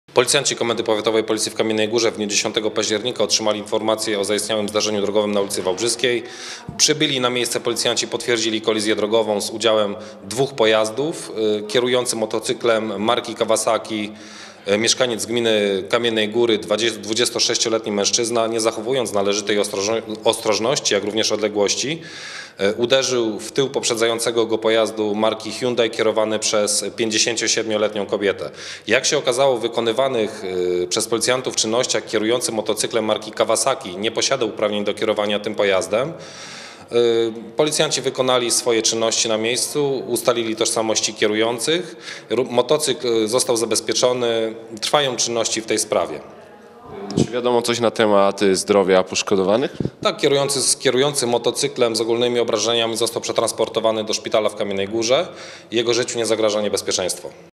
Wypadek motocyklisty w Kamiennej Górze
Kierowca Kawasaki nie posiadał uprawnień do prowadzenia motocykla.